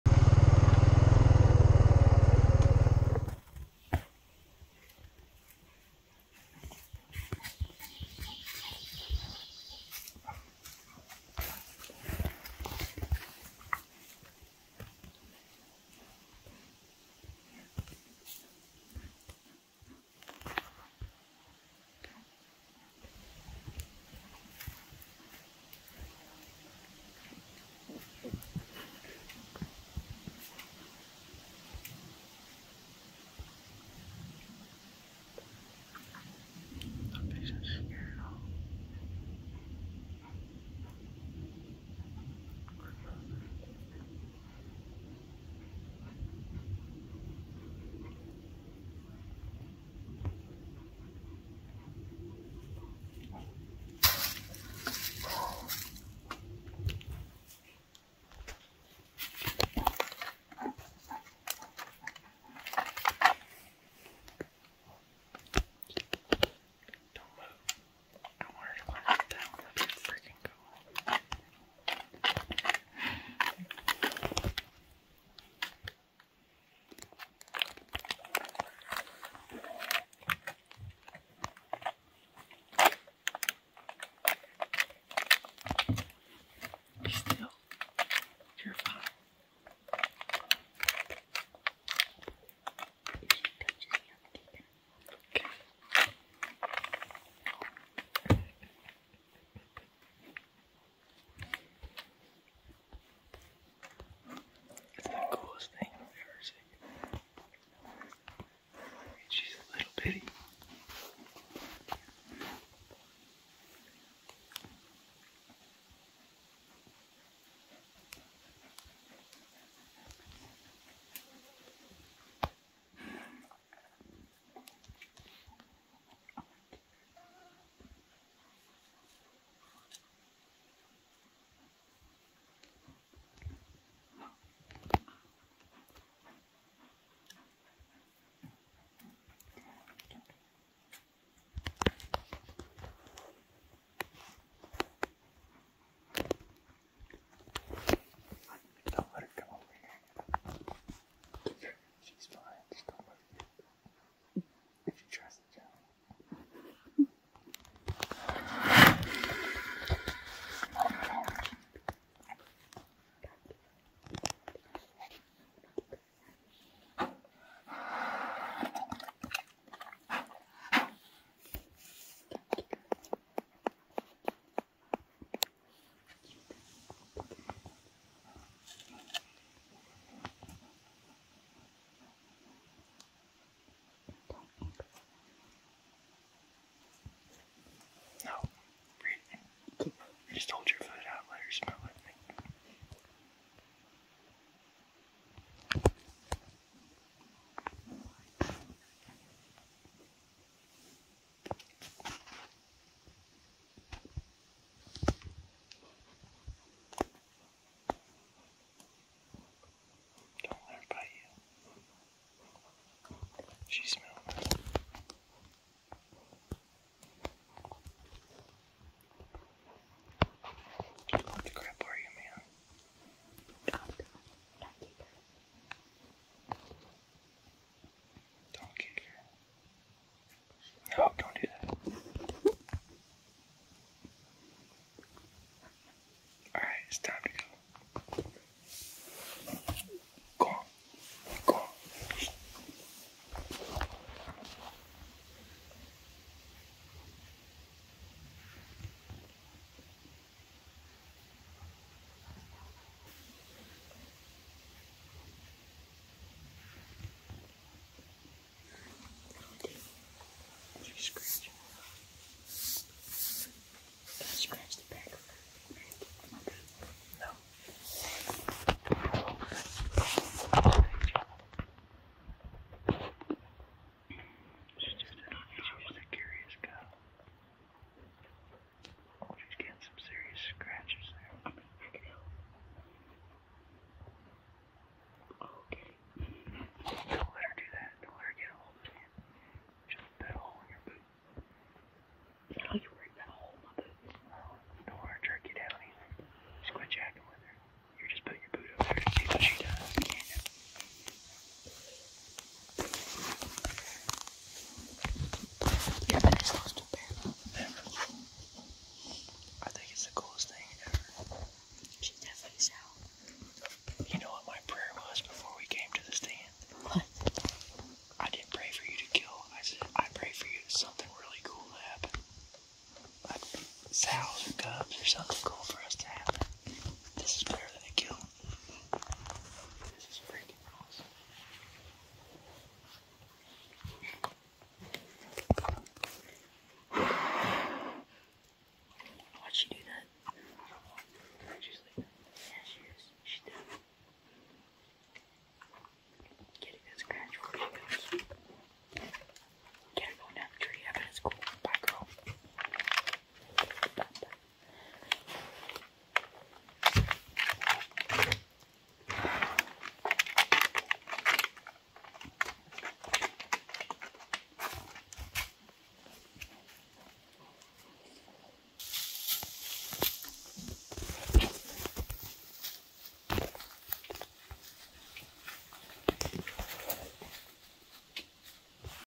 Face to Face with a Bear! INCHES AWAY! Bear licks hunter’s boot!!!
Check out this amazing video of a bear coming face to face with Chase and Rylan White!